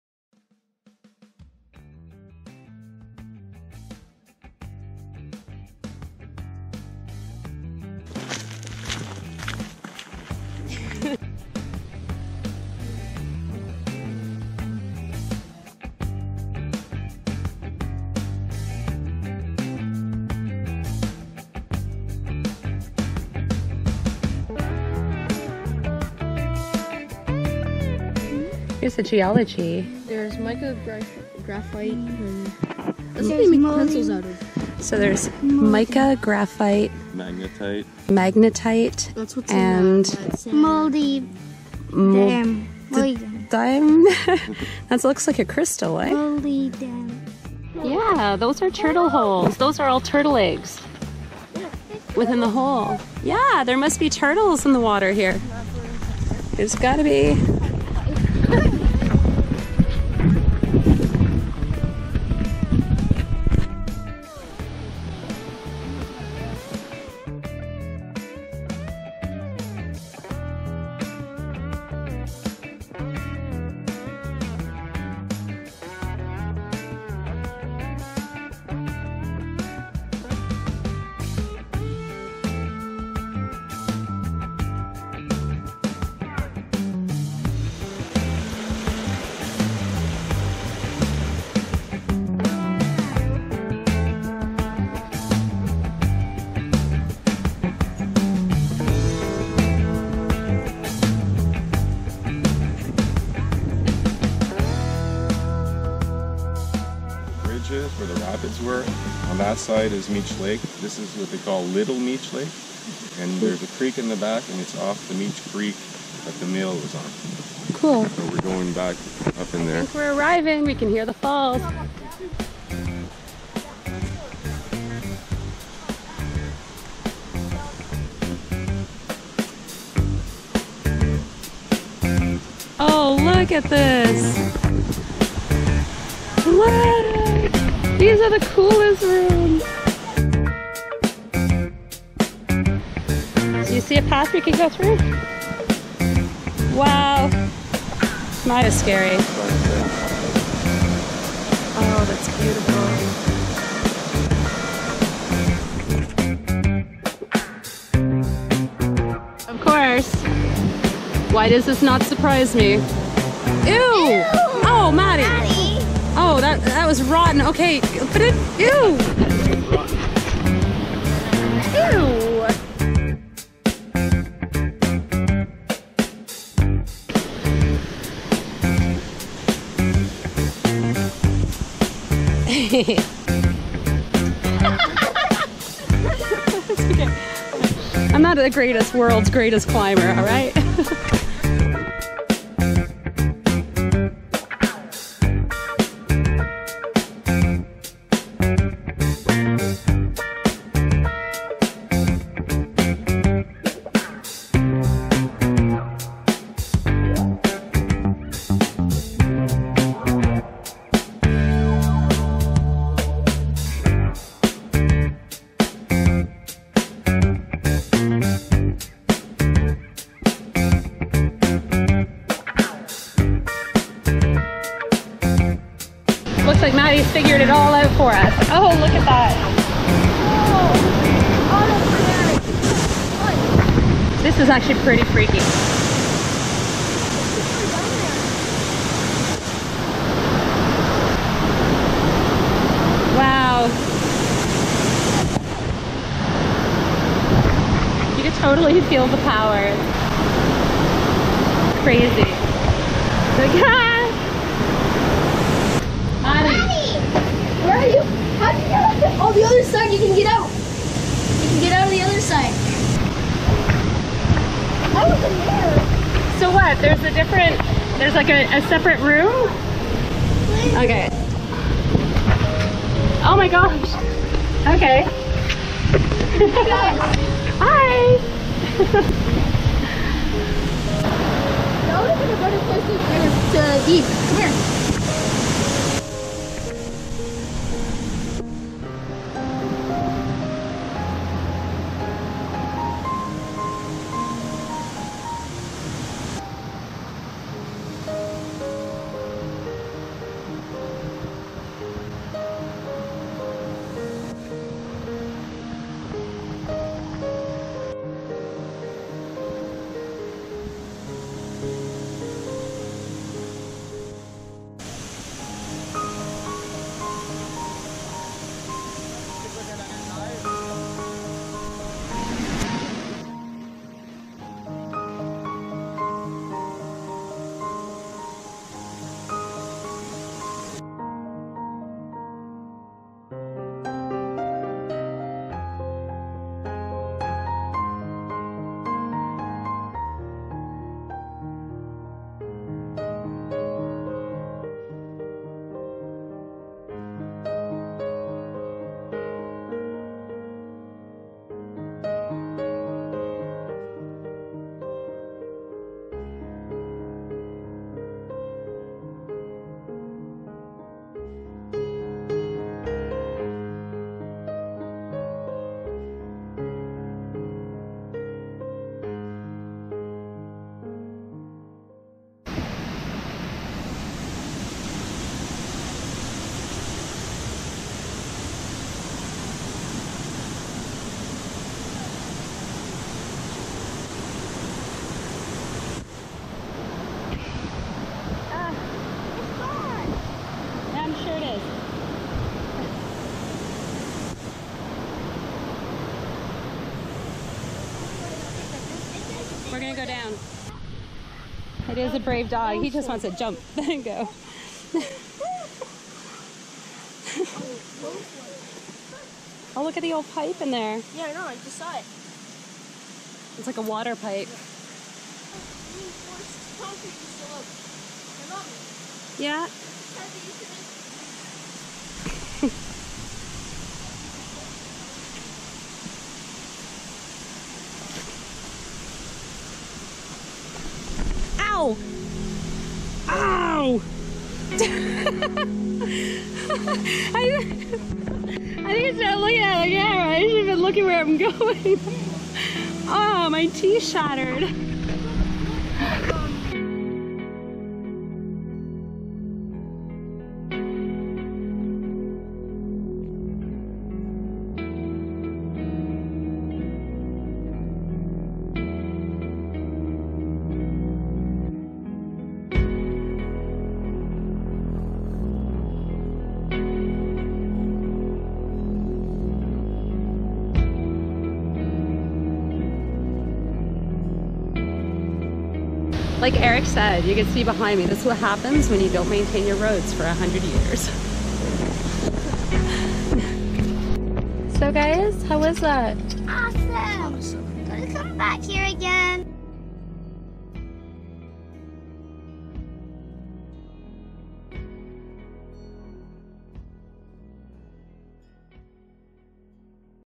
Exploring Carbide Willson Mill Ruins Gatineau Park Canada
Exploring Carbide Mill Ruins in the Gatineau Park Canada. It’s a beautiful place to explore. A nice hike up the beautiful trails leads you to a time that is now left in Ruins. This mill was built by a Canadian inventor Thomas Willson.  People have called him paranoid and even a little crazy. He invented the process to create Calcium Carbide and eventually built his own experimental power station hidden in the Gatineau’s hills. We spent the afternoon exploring the ruins and waterfalls. I was surprised on how much of these ruins still stand today.. A place you must visit if you’re ever nearby!

RDH Metal Detecting
Relic Dirtyhands